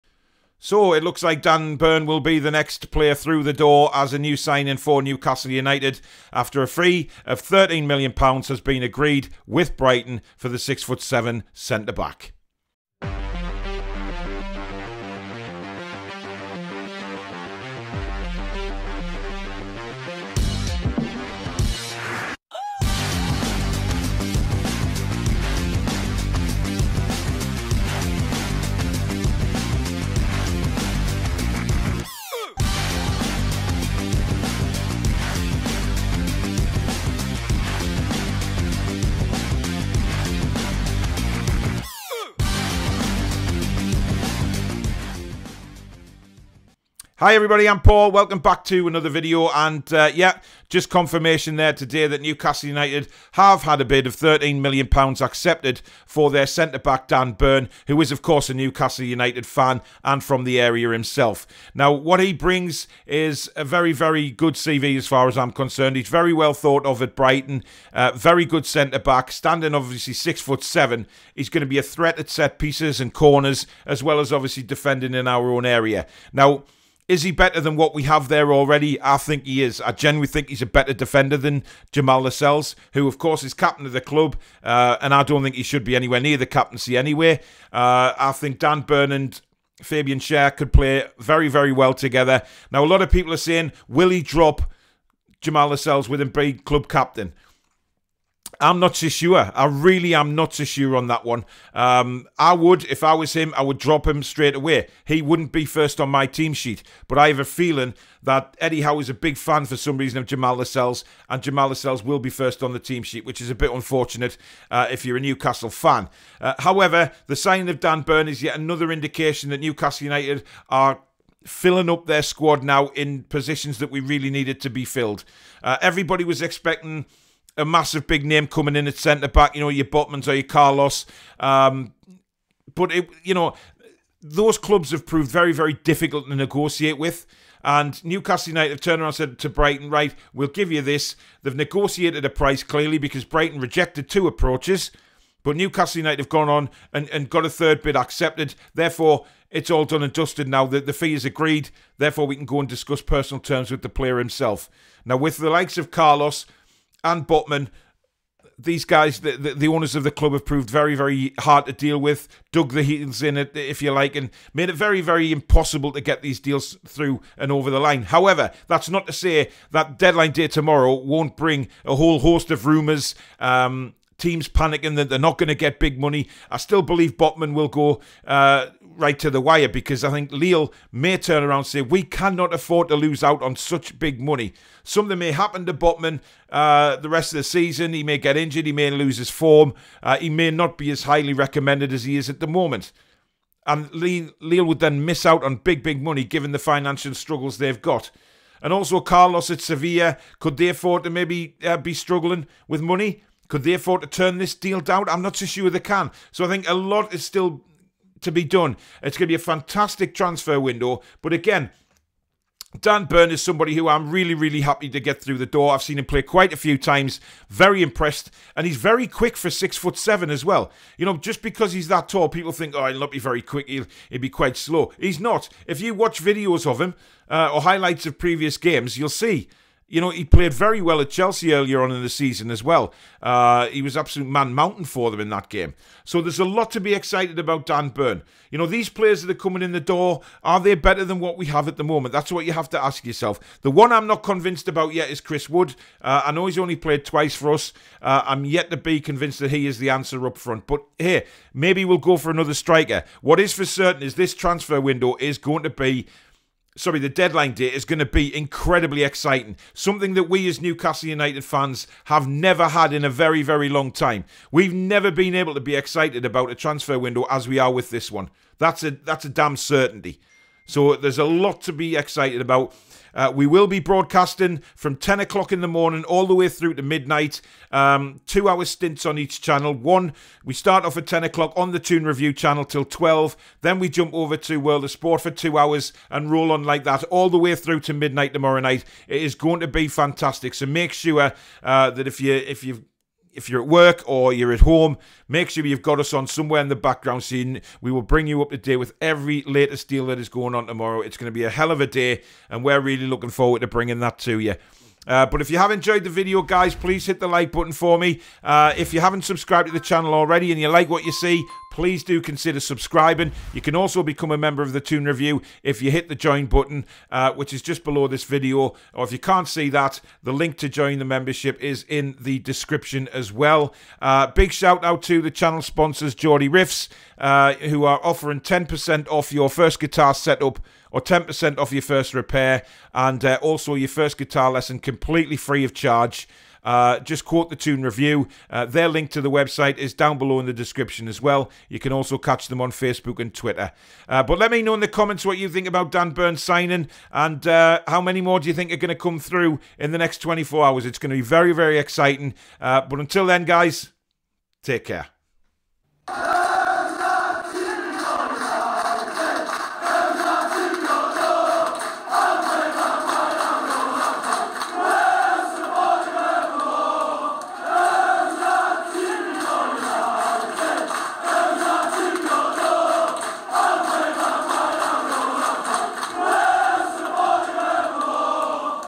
NEWCASTLE UNITED AGREE FEE FOR DAN BURN
There is absolutely no doubt that Newcastle United are going to be one of the major players in the final few hours of the transfer window with Dan Burn from Brighton the next to come through the door pending his medical.

Will Newcastle now turn their attention to a striker?

* First guitar lesson free.
Just quote The Toon Review.